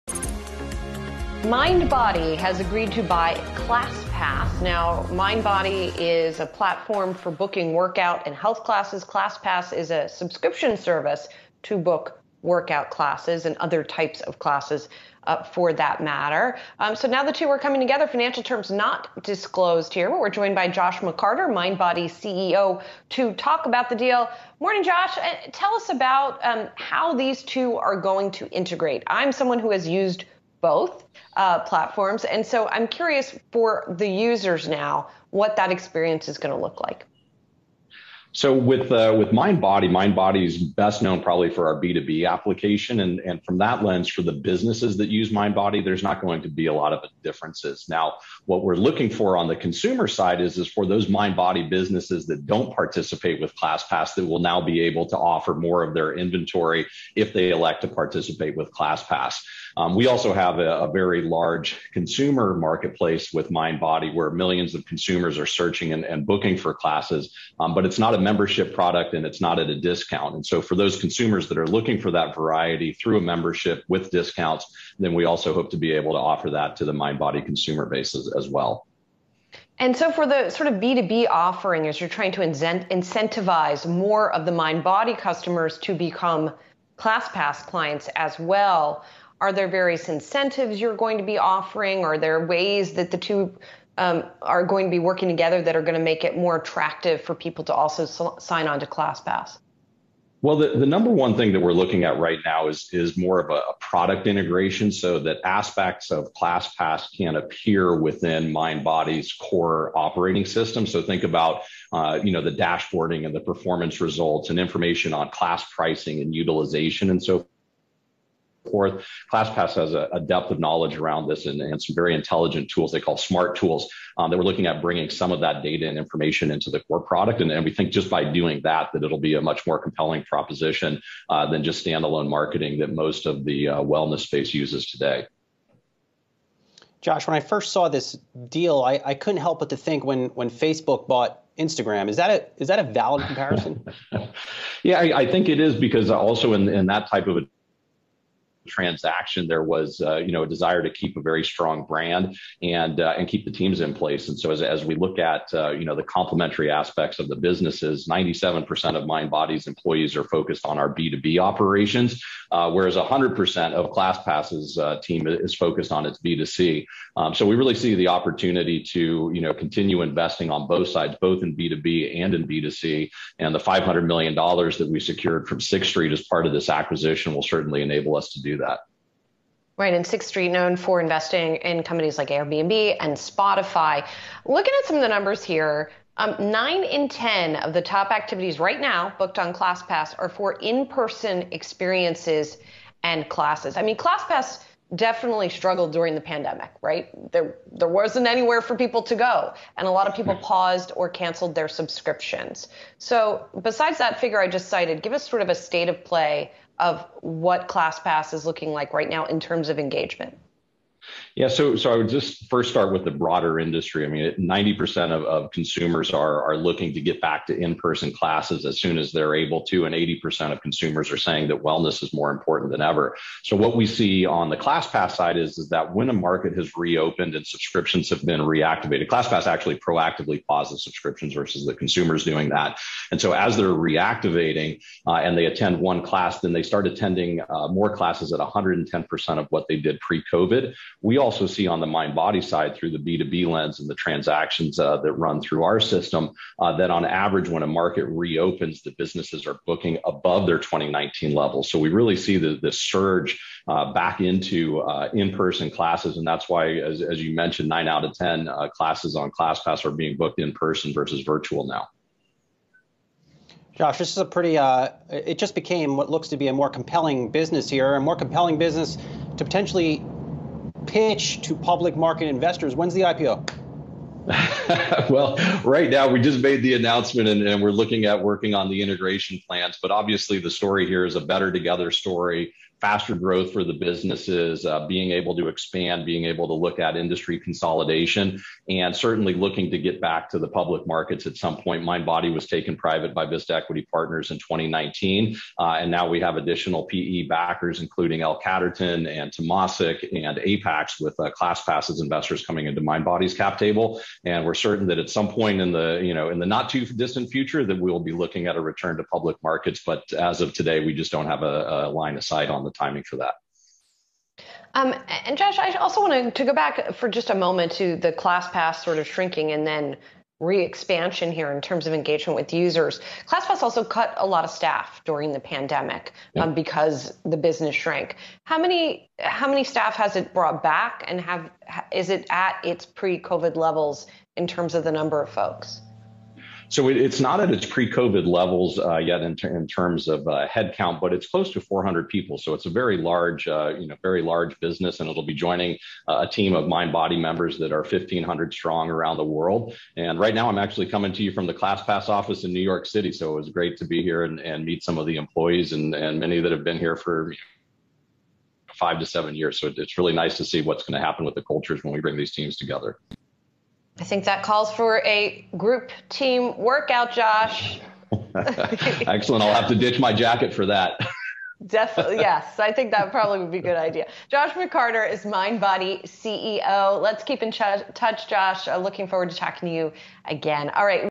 Mindbody CEO on ClassPass acquisition: In-person class bookings are surpassing pre-pandemic levels
Mindbody CEO Josh McCarter joins Yahoo Finance's Julie Hyman to discuss the company's acquisition of ClassPass.